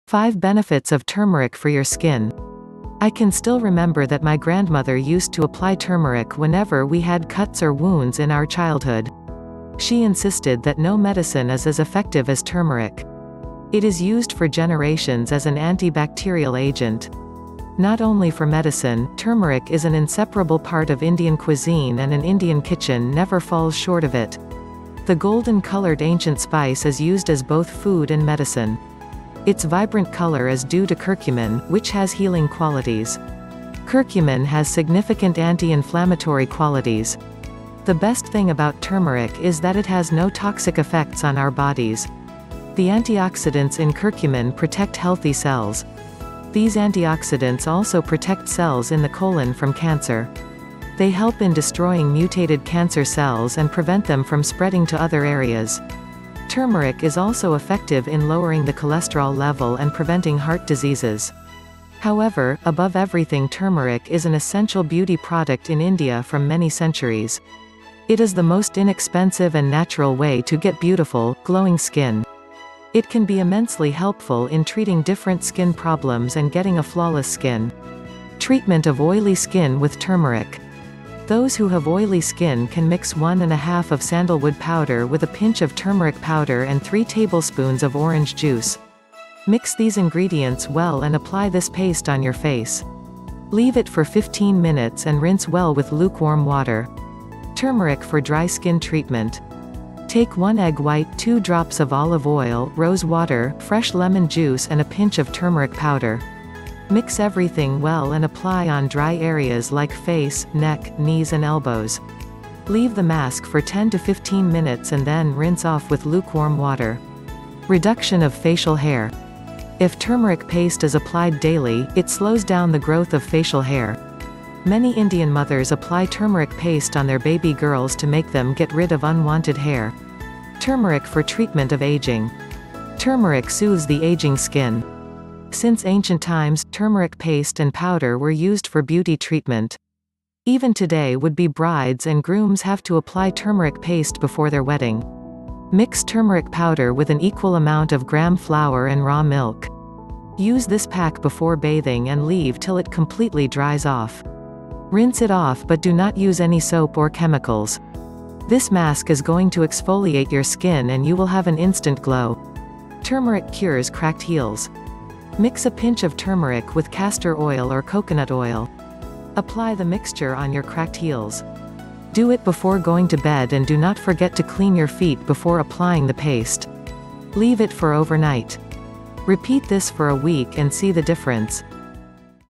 5 benefits of turmeric for your skin
5 benefits of turmeric for your skin-anti bacterial agent, turmeric has cucumin for healing qaulities, has no toxic effects for our body ,antioxidants for healthy cells, lowering the cholesterol levels preventing heart disease. essential for beauty products, treating skin problems make beautiful and glowing skin soothes aging skin and cures cracked heels.